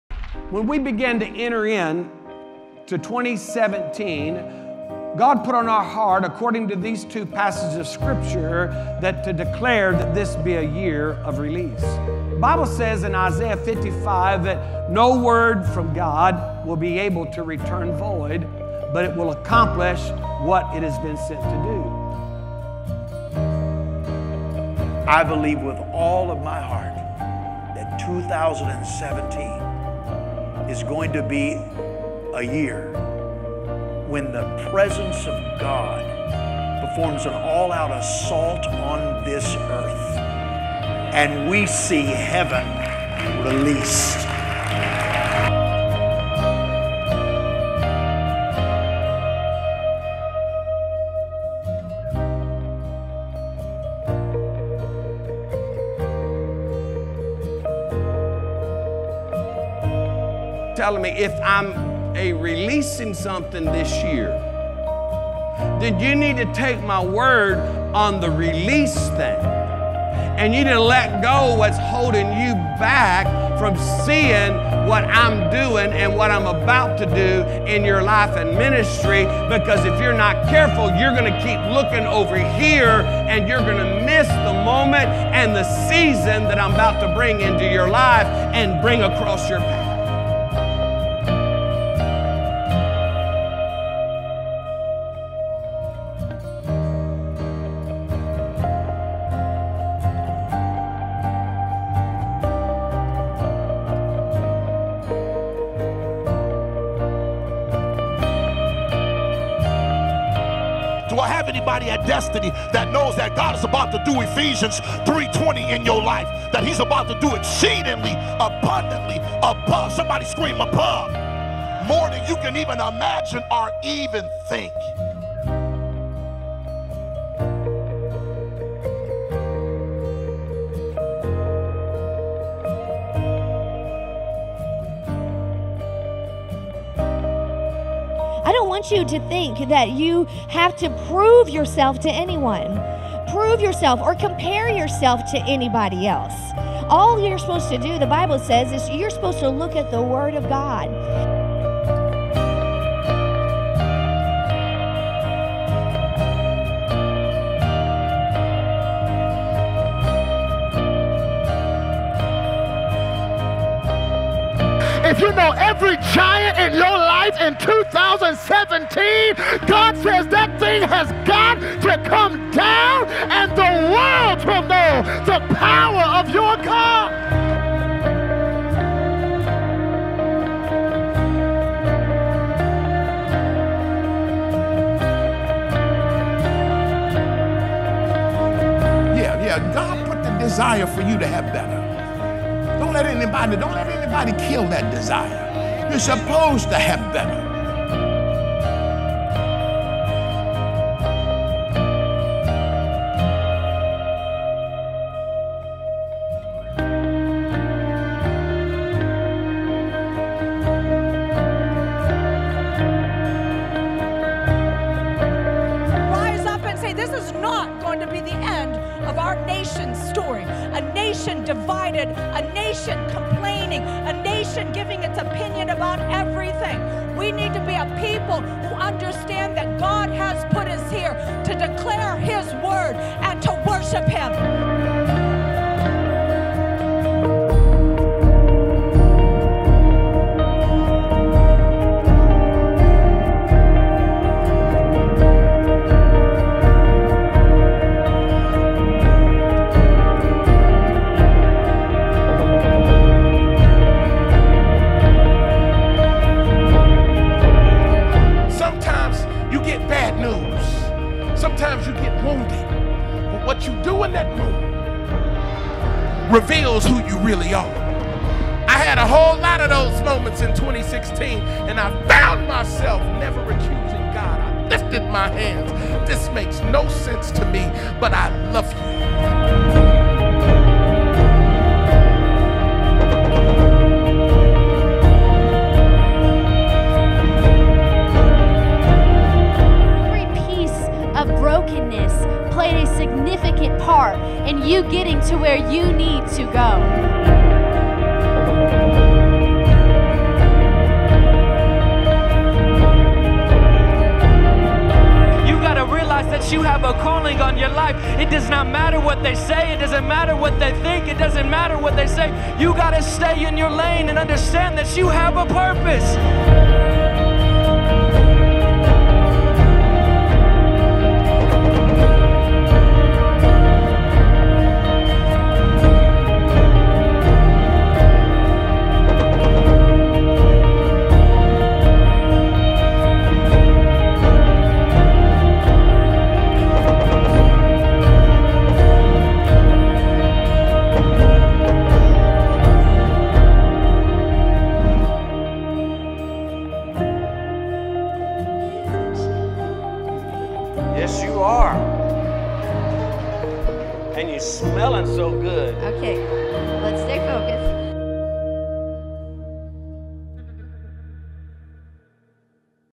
DWOC 2017 recap
A recap of just a few of the things that happened at Destiny this year. We had some amazing speakers an accomplished so much, 2018 will be above and beyond.